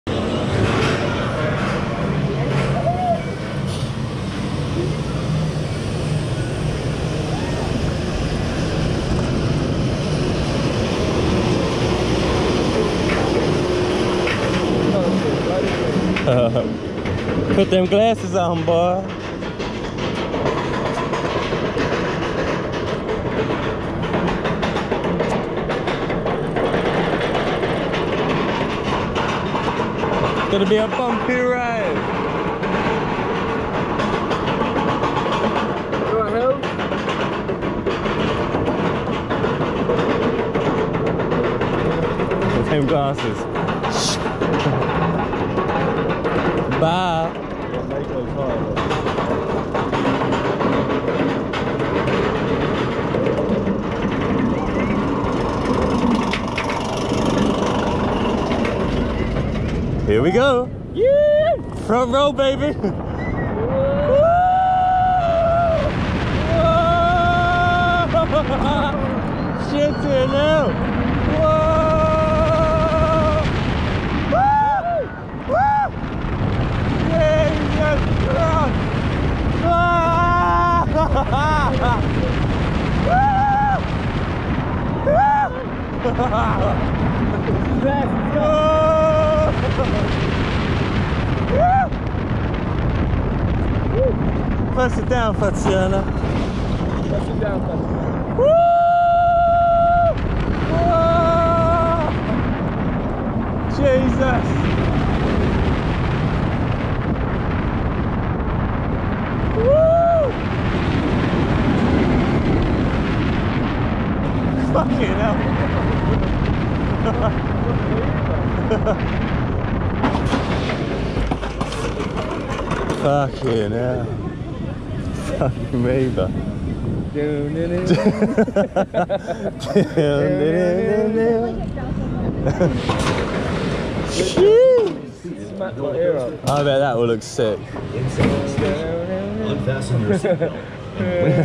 Kraken front seat 4K POV SeaWorld Orlando (GOPRO 7 HYPERSMOOTH 50fps)
Join me for a POV ride on Kraken at SeaWorld Orlando, Florida.

Shot on Gopro 7 Black Hypersmooth